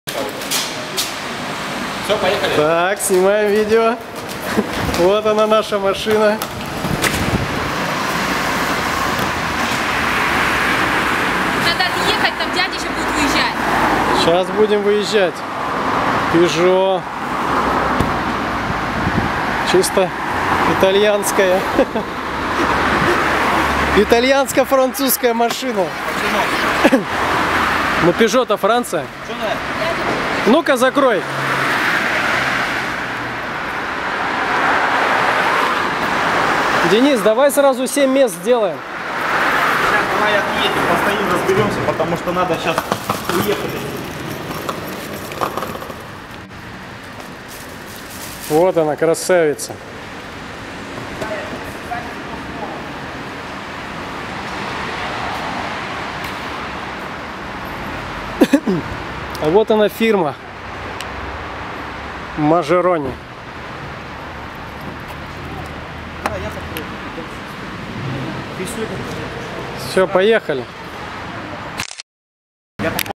Аренда авто в Италии
Заранее забронированный автомобиль онлайн получаем на выдаче в Турине
Все видео канала Путешествуем с детьми:

7-ми местный Пежо (Peugeot) 5008 в нашем распоряжении на 10 дней.
Маршрут на автомобиле с детьми из Турина в Рим начинается